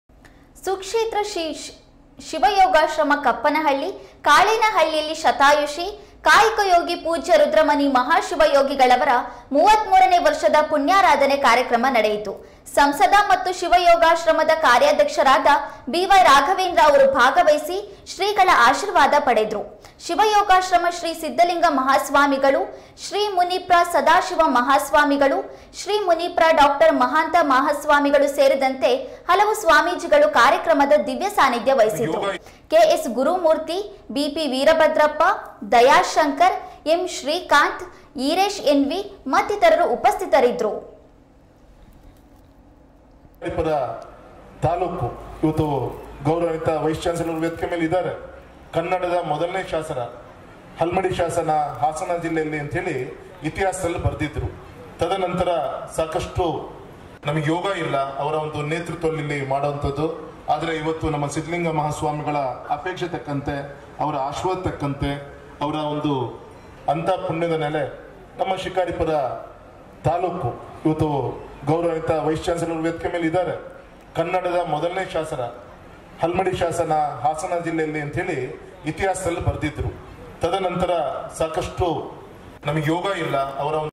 ಶತಾಯುಷಿ, ಕಾಯಕ ಯೋಗಿ ಪೂಜ್ಯ ರುದ್ರಮುನಿ ಮಹಾಶಿವಯೋಗಿಗಳವರ 33ನೇ ವರ್ಷದ ಪುಣ್ಯಾರಾಧನೆ
ಸುಕ್ಷೇತ್ರ ಶ್ರೀ ಶಿವಯೋಗಾಶ್ರಮ ಕಪ್ಪನಹಳ್ಳಿ - ಕಾಳೇನಹಳ್ಳಿಯಲ್ಲಿ ಶತಾಯುಷಿ, ಕಾಯಕ ಯೋಗಿ ಪೂಜ್ಯ ರುದ್ರಮುನಿ ಮಹಾಶಿವಯೋಗಿಗಳವರ 33ನೇ ವರ್ಷದ ಪುಣ್ಯಾರಾಧನೆ ಕಾರ್ಯಕ್ರಮ ನಡೆಯಿತು. ಸಂಸದ ಮತ್ತು ಶಿವಯೋಗಾಶ್ರಮದ ಕಾರ್ಯಾಧ್ಯಕ್ಷರಾದ ಬಿ.ವೈ.ರಾಘವೇಂದ್ರ ಅವರು ಭಾಗವಹಿಸಿ ಶ್ರೀಗಳ ಆಶೀರ್ವಾದ ಪಡೆದರು. ಶಿವಯೋಗಾಶ್ರಮ ಶ್ರೀ ಸಿದ್ದಲಿಂಗ ಮಹಾಸ್ವಾಮಿಗಳು, ಶ್ರೀ. ಮ.ನಿ.ಪ್ರ ಸದಾಶಿವ ಮಹಾಸ್ವಾಮಿಗಳು, ಶ್ರೀ. ಮ.ನಿ.ಪ್ರ. ಡಾ. ಮಹಾಂತ ಮಹಾಸ್ವಾಮಿಗಳು ಸೇರಿದಂತೆ ಹಲವು ಸ್ವಾಮೀಜಿಗಳು ಕಾರ್ಯಕ್ರಮದ ದಿವ್ಯ ಸಾನ್ನಿಧ್ಯ ವಹಿಸಿದ್ದರು. ಕೆ.ಎಸ್.ಗುರುಮೂರ್ತಿ, ಬಿ.ಪಿ.ವೀರಭದ್ರಪ್ಪ, ದಯಾಶಂಕರ್, ಎಂ.ಶ್ರೀಕಾಂತ್, ಈರೇಶ್ ಎನ್.ವಿ. ಮತ್ತಿತರರು ಉಪಸ್ಥಿತರಿದ್ದರು.